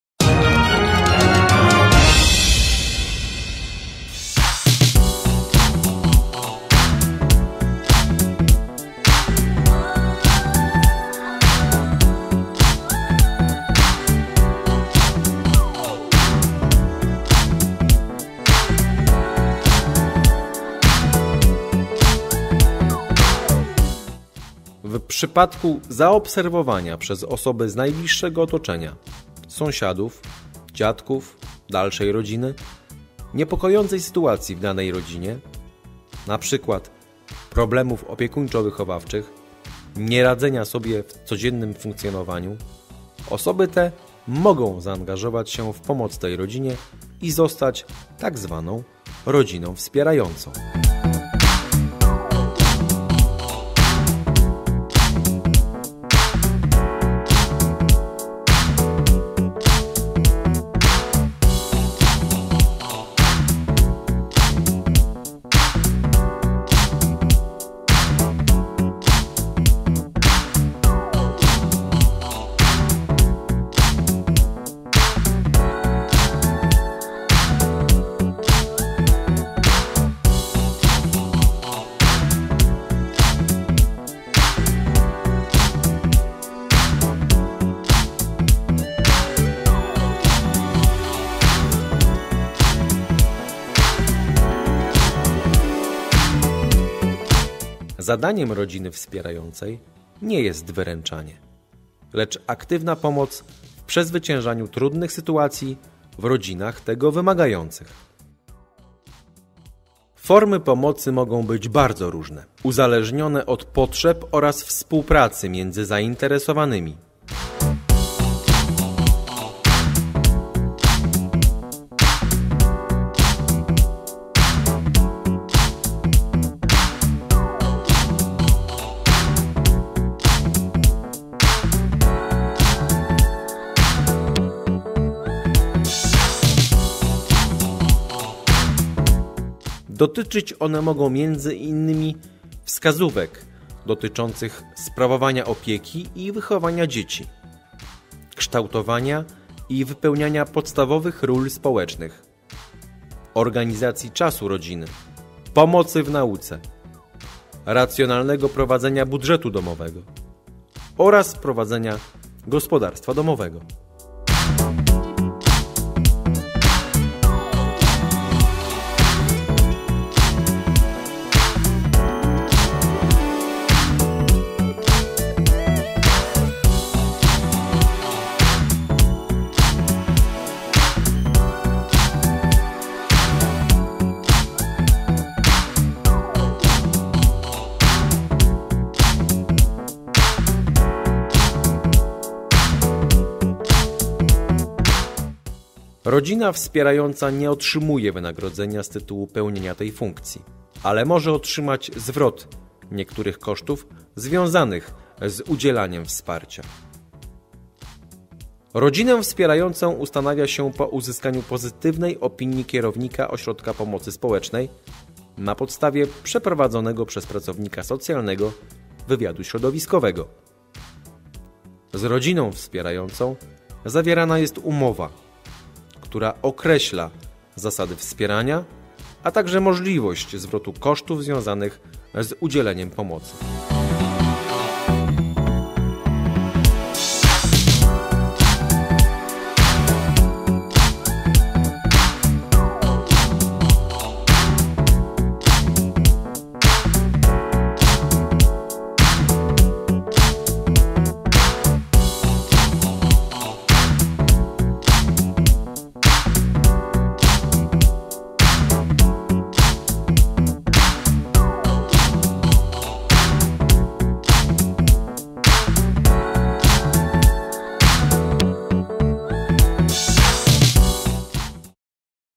MOPS Biała Podlaska: Poszukiwana rodzina wspierająca
Miejski Ośrodek Pomocy Społecznej poszukuje chętnych do programu rodzin wspierających. Więcej informacji w materiale wideo.